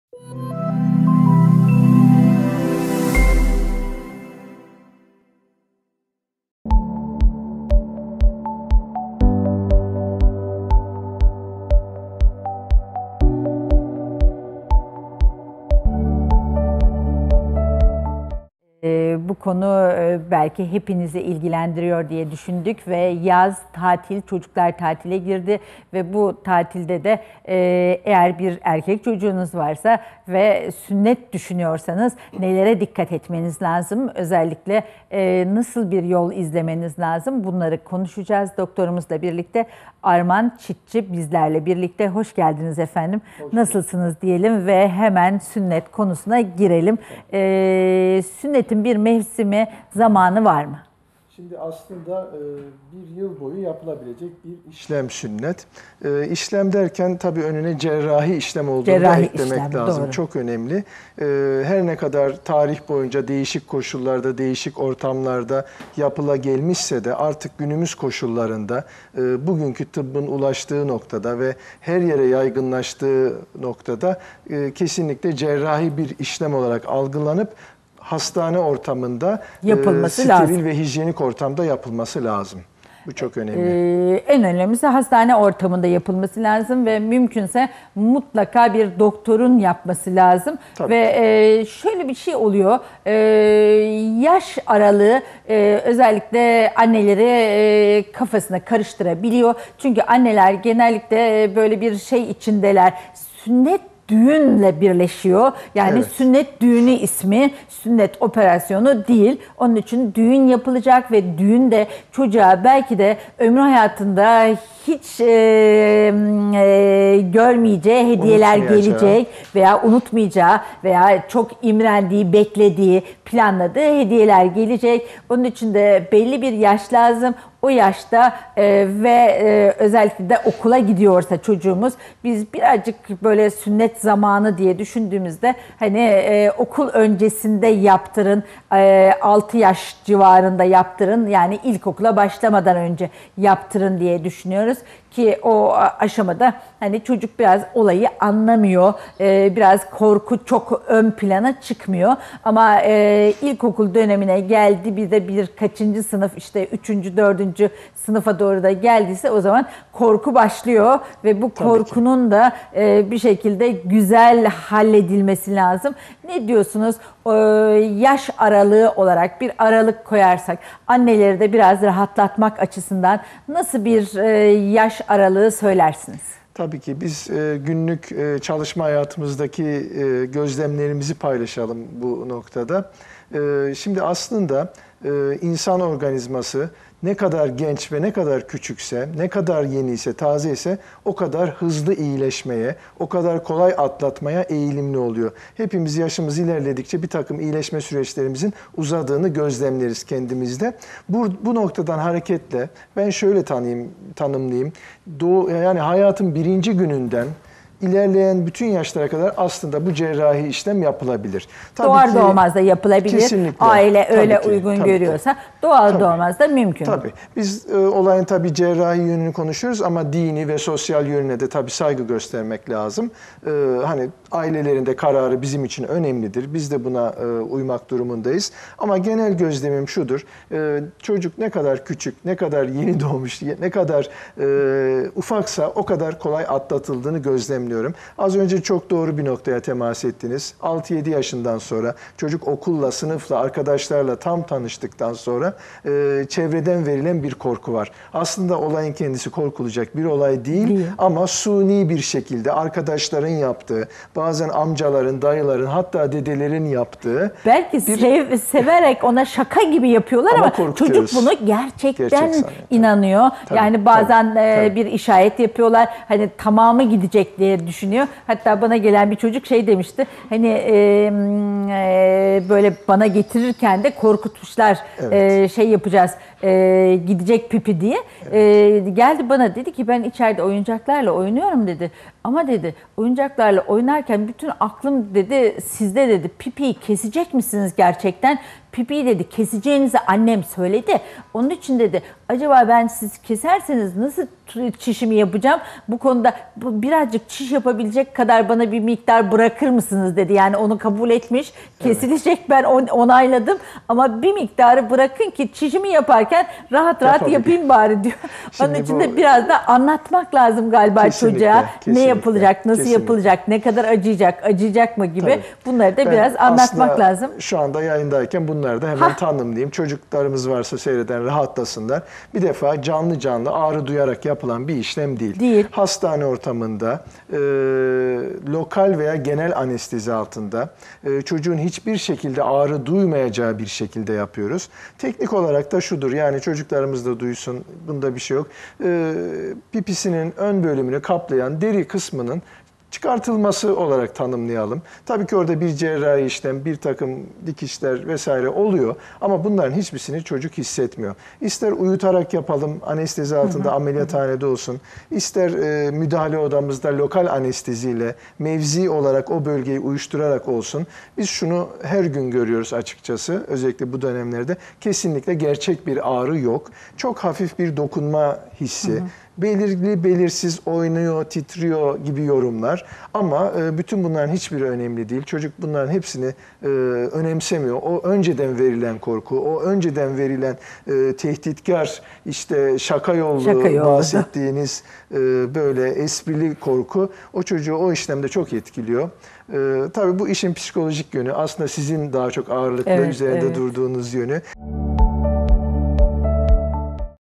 Sünnette Nelere Dikkat Edilmesi Gerekir?
Avrasya Hastanesi Üroloji Uzmanı Op. Dr. Arman Çitçi, sünnette nelere dikkat edilmesi gerektiğini belirtiyor.